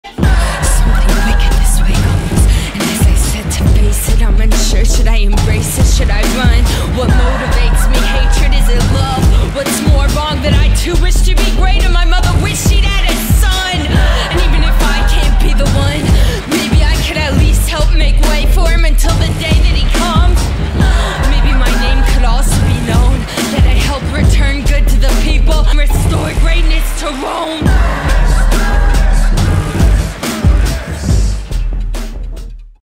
BRUTUS- finished ych
Hi guys!!! Sorry about the lack of activity, I´ve been quite ill, a lot better now. Anyways, here is the finished pmv for @puffanimates , enjoy!!!